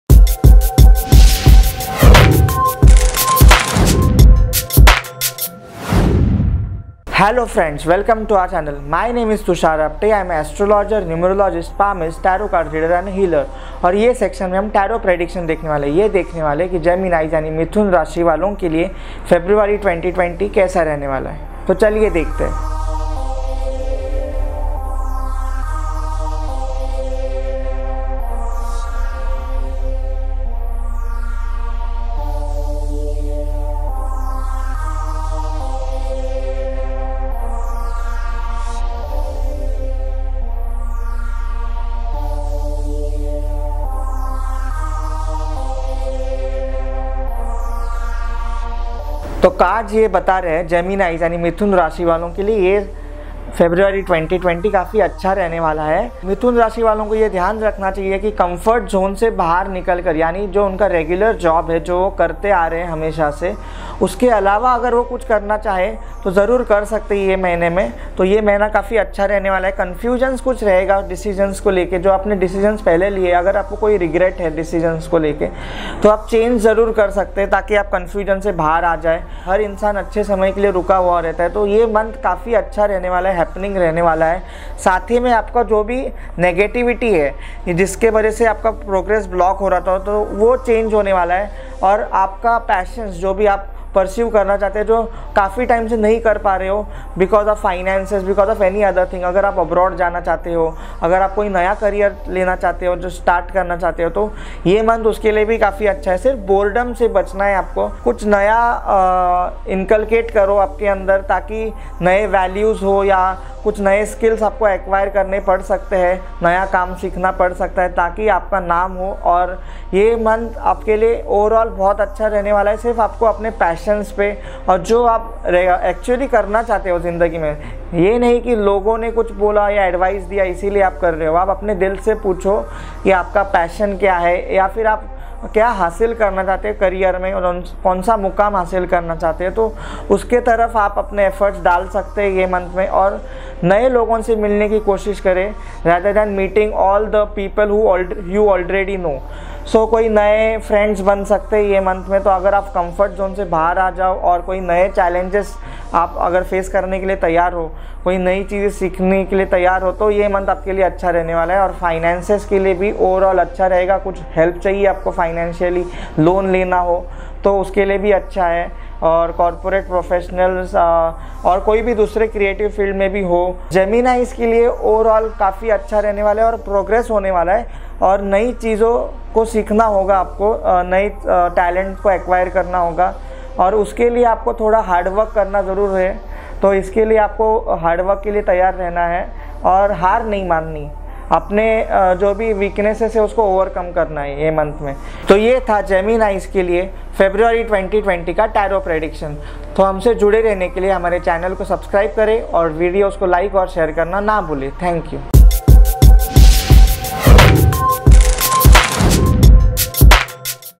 Gemini: Don't Run From Hard Work - भूल कर भी मेहनत से जी ना चुराना
F3 Studioz presents Mr. Tushar Apte. A famous  Astrologer, Numerologist Tarot Reader, Palmist & Healer. Watch out this exclusive video for Gemini Zodiac Sign's February 2020 Tarot Predictions.

0:00:06.820,0:00:08.780

0:00:08.780,0:00:13.660
My name is Tushar Apte. I am an astrologer, numerologist, palmist, tarot card reader and a healer

0:00:13.660,0:00:15.880
In this section we are going to see Tarot Predictions

0:00:16.000,0:00:21.260
We are going to se how February 2020 is going to be for Gemini (Mithun)

0:00:21.340,0:00:22.540

0:00:49.080,0:00:56.220
As per the cards, February 2020 is going to be very good for Gemini (Mithun)

0:00:56.320,0:01:05.940
If you want to come out of your comfort zone and try to do something new apart from your routine job

0:01:05.940,0:01:09.780
Then this is the right month for you

0:01:09.780,0:01:15.520
There will be some confusions and in case you have any regret for the decision you have taken in past

0:01:15.580,0:01:19.420
Then you can change it so that you can come out of confusion

0:01:19.420,0:01:24.340
As everybody waits for the right time, so this month is going to be happening for you

0:01:24.500,0:01:31.520
The negativity which was blocking your progress, is going to be change

0:01:31.680,0:01:39.040
If you have any passion and you were not able to pursue it from so long because of finance or any other thing

0:01:39.040,0:01:46.400
If you want to go abroad, want to switch to any new career then this is the right time for the same

0:01:46.400,0:01:51.780
Stay away from boredom and inculcate something new in you

0:01:51.880,0:01:58.940
You may require to acquire new skills, learn new work so that you will be famous

0:01:59.080,0:02:07.660
Overall this is a good month for you, just follow your passion or do something which you actually want to do

0:02:07.740,0:02:15.140
Listen to your heart, your interest, your passion instead following others

0:02:15.360,0:02:23.220
Understand what you want to achieve in your life and accordingly put your efforts in that direction

0:02:23.460,0:02:29.420
Try to meet new people rather than meeting the people you already know

0:02:29.520,0:02:38.920
New friends are on the cards, if you are willing to come out of the comfort zone and learn something new

0:02:38.940,0:02:44.000
Then its a good month and even its good for the finances

0:02:44.180,0:02:49.000
If you are looking for financial help, loan then its good for that too

0:02:49.000,0:02:56.500
Whether you belong to corporate sector & creative filed, this month is going to be very good for Gemini

0:02:56.580,0:03:03.140
Lot of progress is on the cards. Just learn new things and acquire new talent

0:03:03.340,0:03:10.880
For that hard work is must so be ready for the hard work and never give up

0:03:11.020,0:03:14.800
Try to overcome your weaknesses in this month

0:03:14.980,0:03:19.600
So, that was the Tarot Prediction for Gemini (Mithun) for February 2020

0:03:19.600,0:03:22.380

0:03:22.380,0:03:25.260